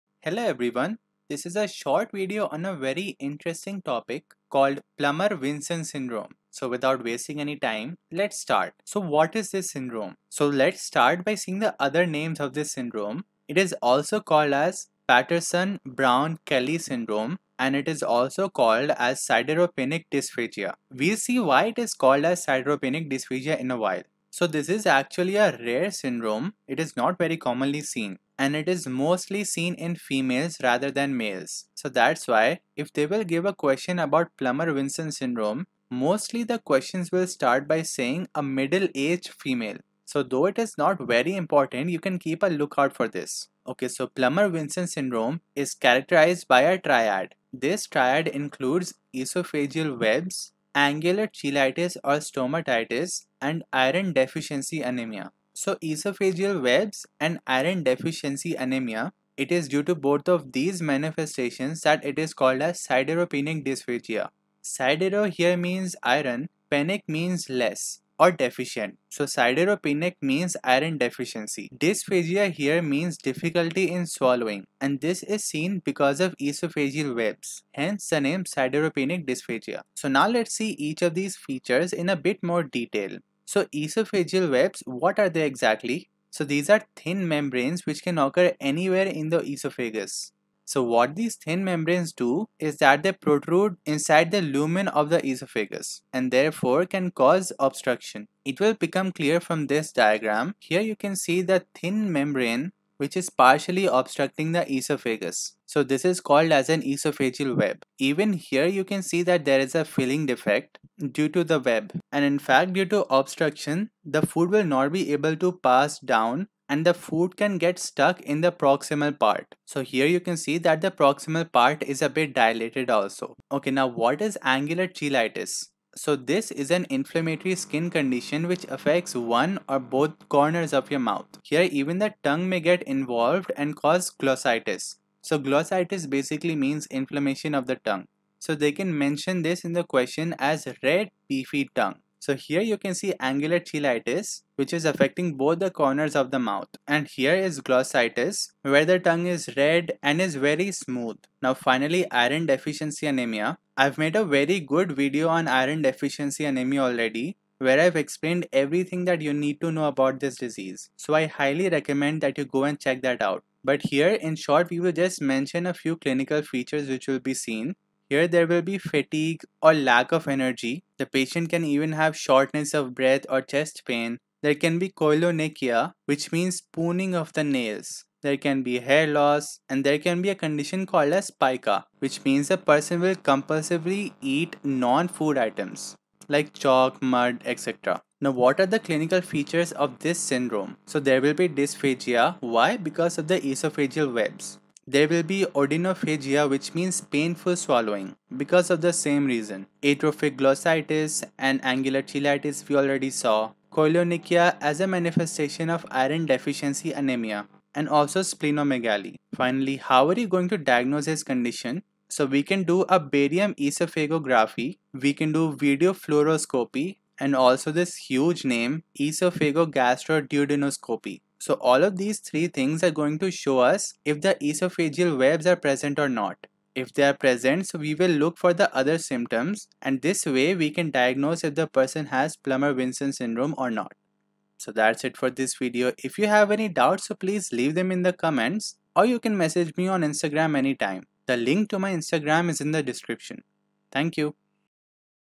Plummer Vinson Syndrome | Triad, Clinical Features, Diagnosis | NEET-PG/NExT
Visit my Instagram for previous year questions and memory hacks!